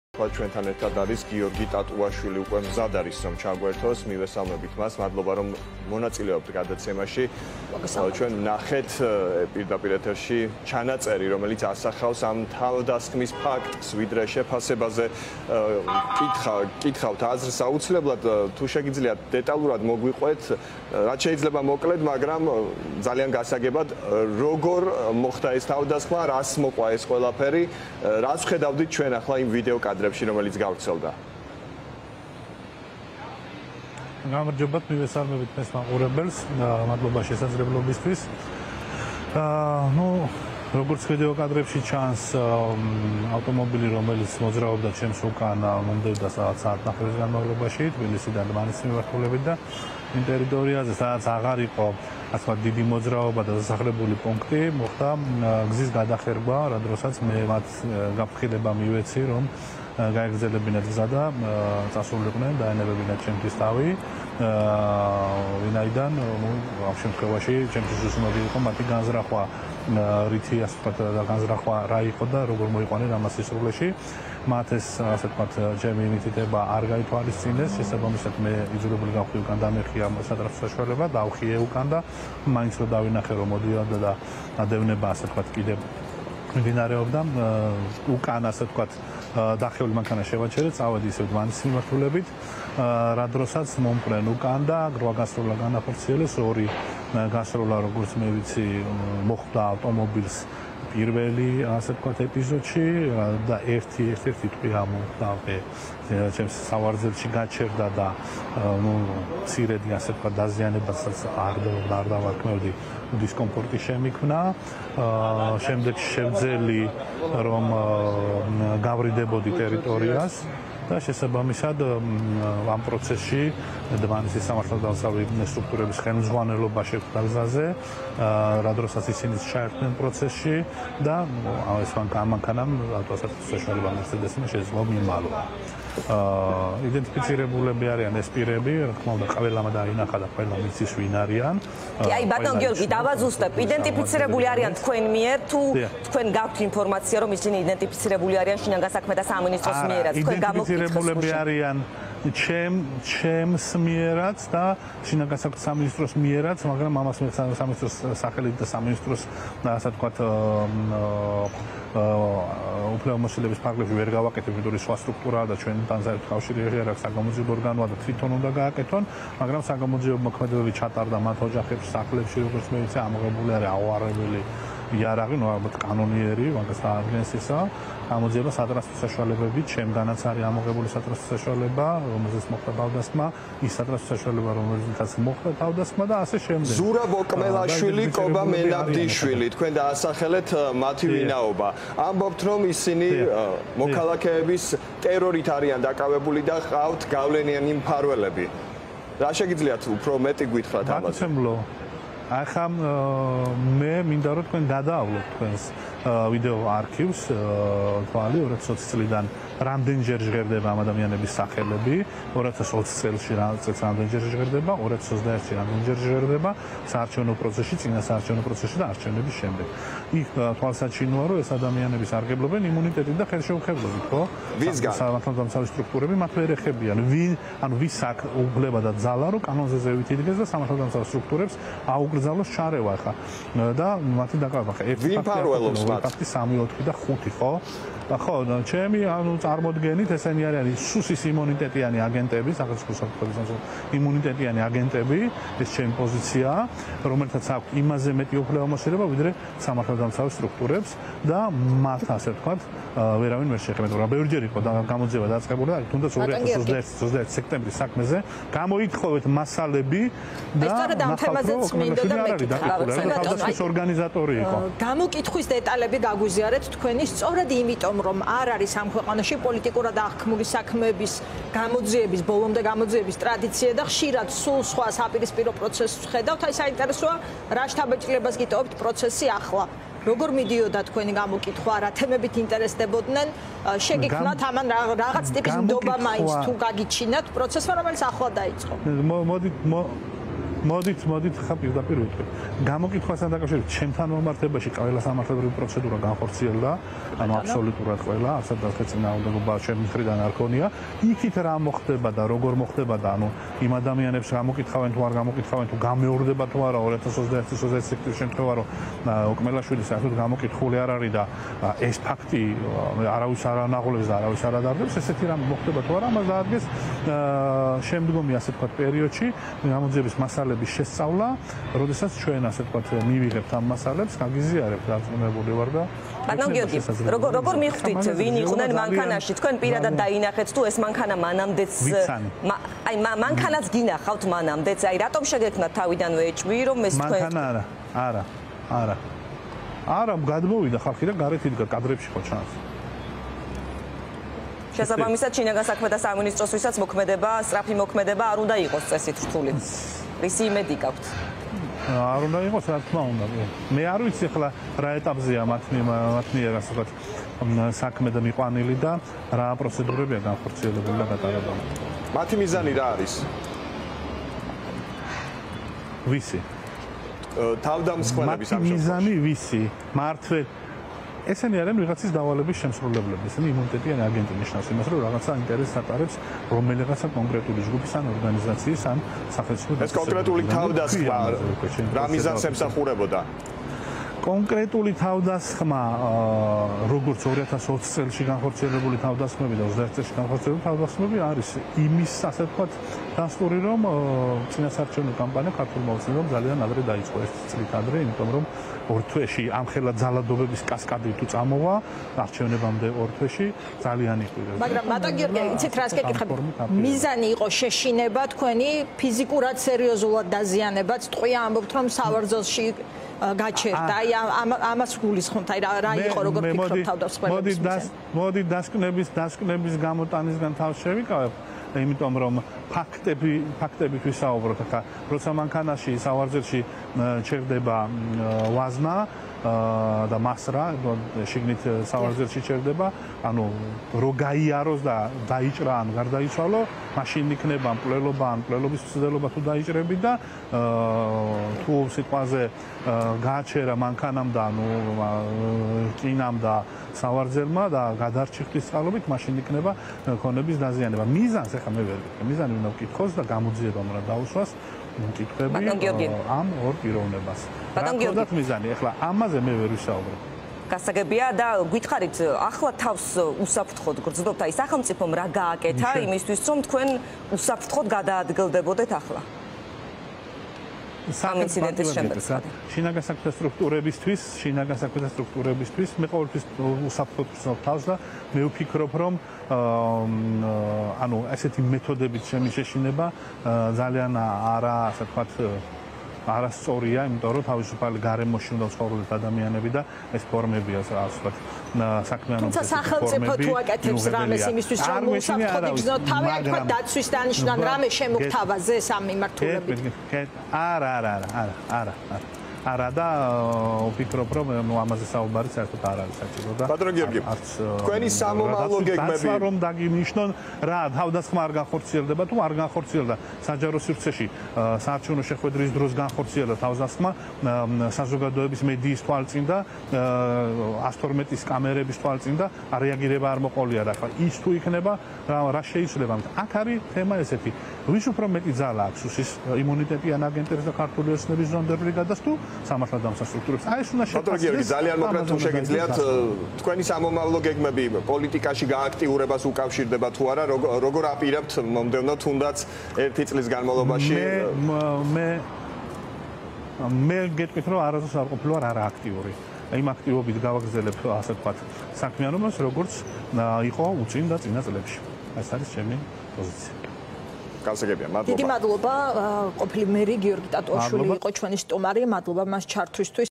იმ აქტივობით გავაგრძელებ საქმიანობას, როგორიც მქონდა წინა წლებში — ტატუაშვილი
▪️ „ეს თავდასხმა არის იმის დასტური, რომ წინასაარჩევნო კამპანია „ოცნებამ“ ერთი წლით ადრე დაიწყო“ — გიორგი ტატუაშვილი, დმანისის ყოფილი მერი